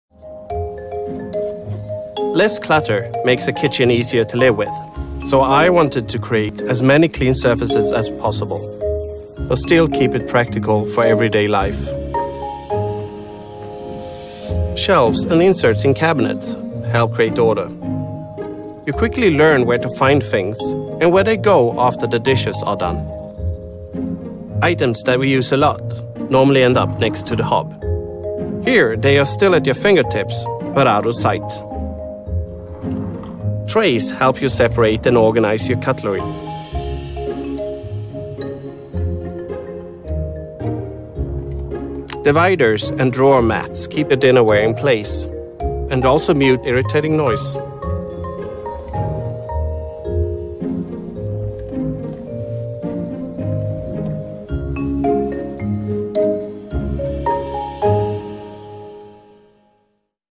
IKEA 檯面清爽 思緒更清晰
整齊有序的廚房令人賞心悅目，每件物品妥善收納又能隨手取得，讓廚房運作更輕鬆。